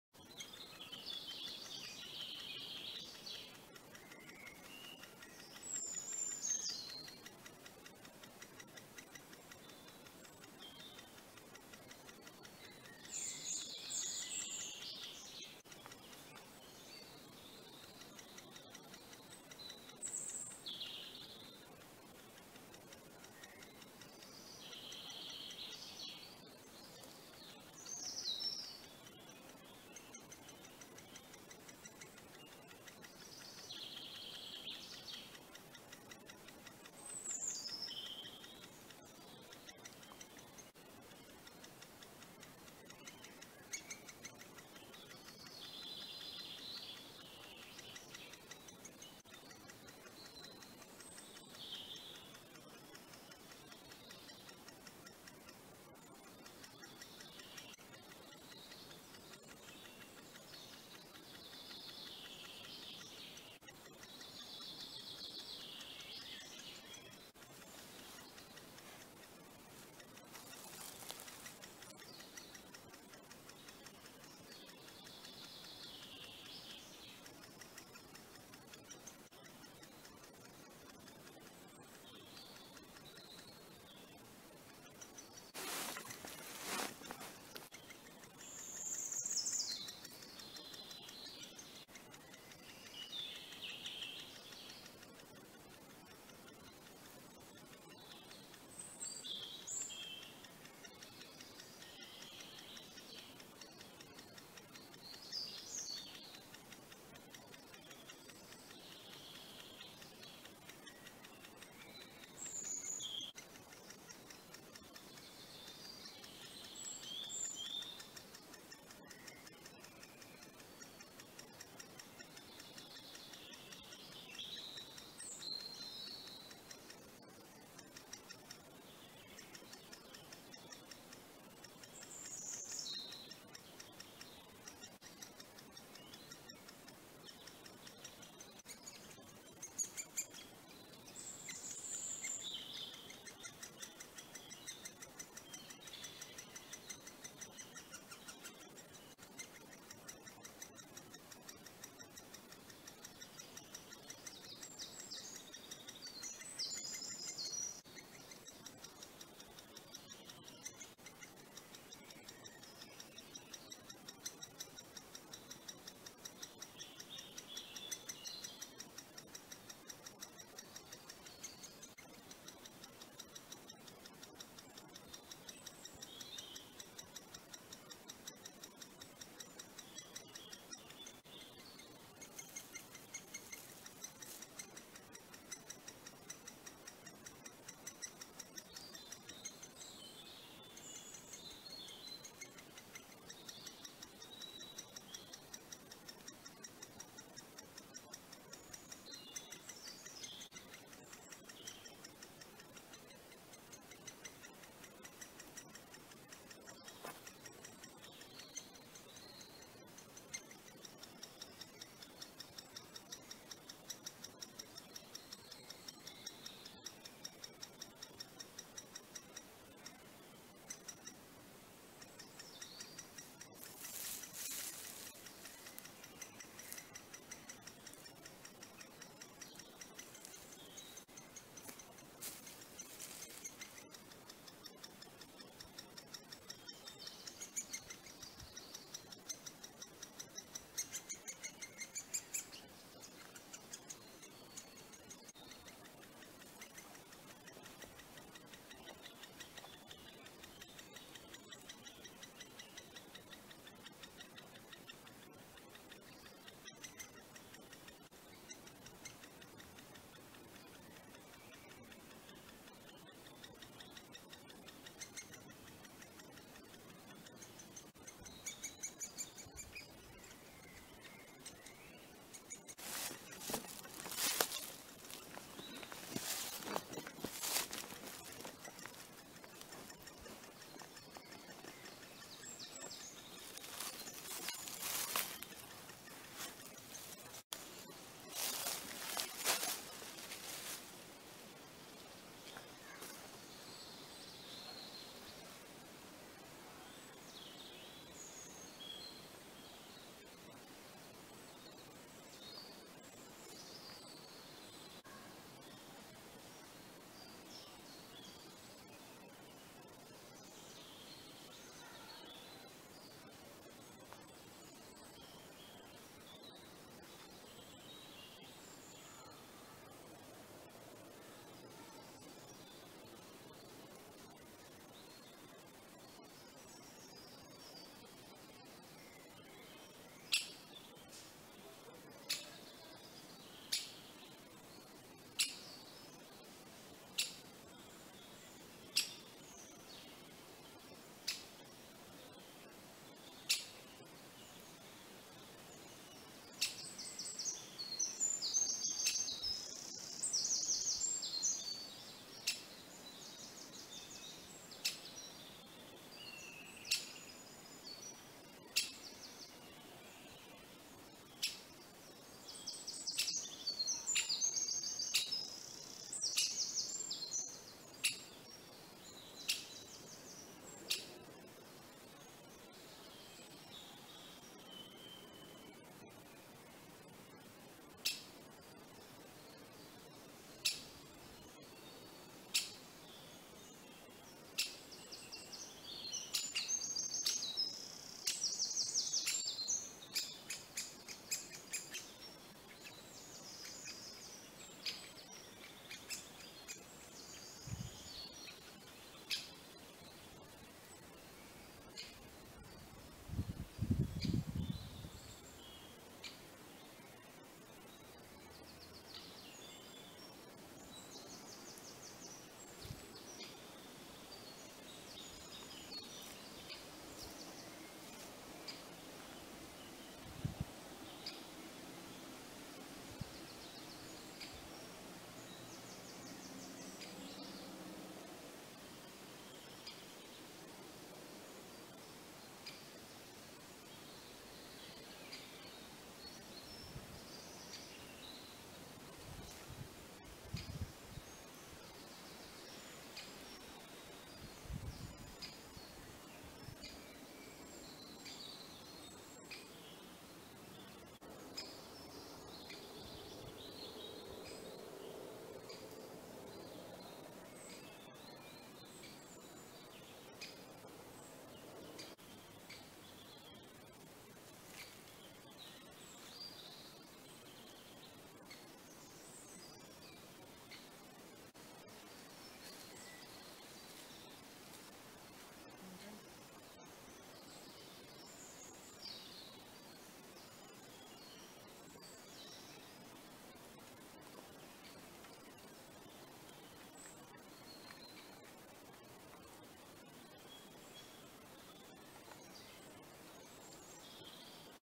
Great spotted woodpecker nest
Great spotted woodpecker nest. filmed late may 2020 in  the north of england